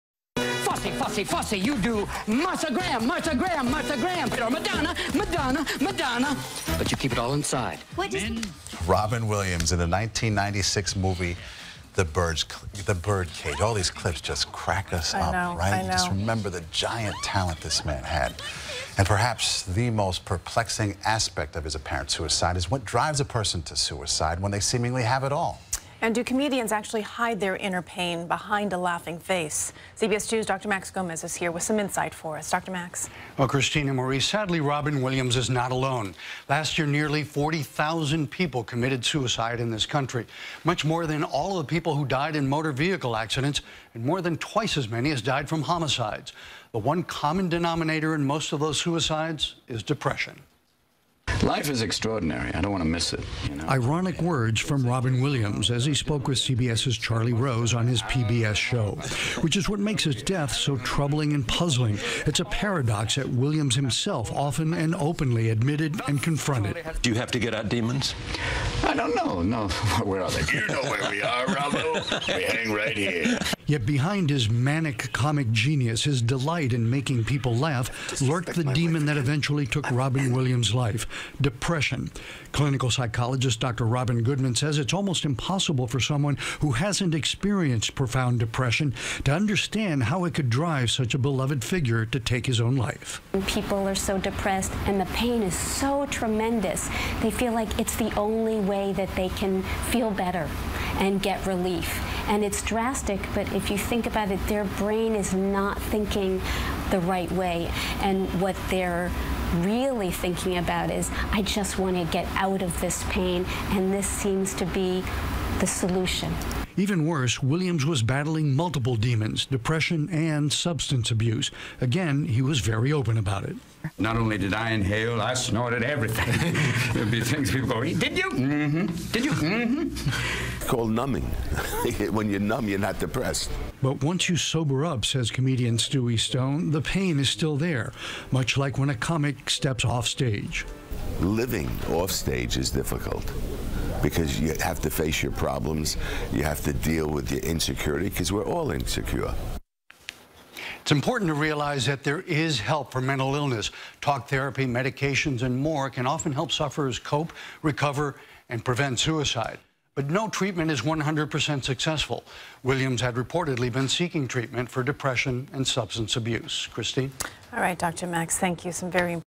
Dr. Max Gomez: Understanding Depression
Many people are perplexed by what could drive a man with the world seemingly at his feet to end his life. CBS 2's Dr. Max Gomez has some insight.